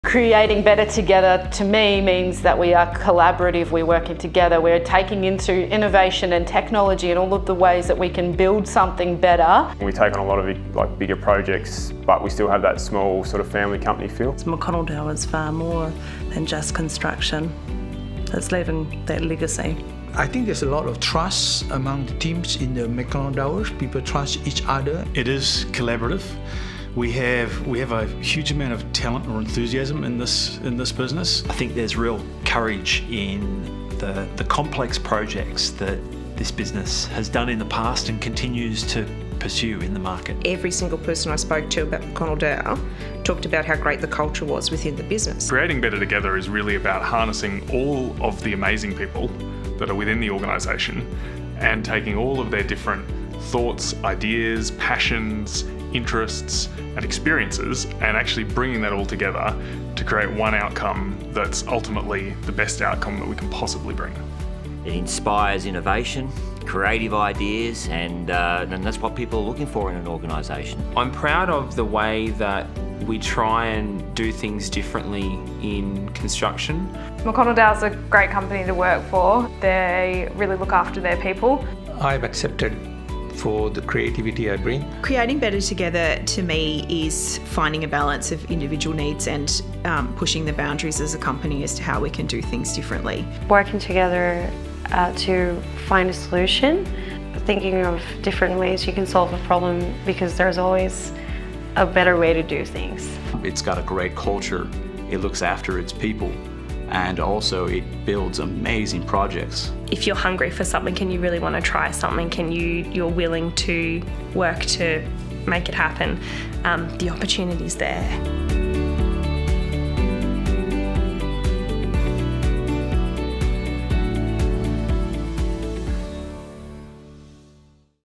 Creating Better Together
You are part of building something special at McConnell Dowell.
Creating better together for each other, our customers and communities.
Construct a great career delivering projects that shape our cities and improve lives.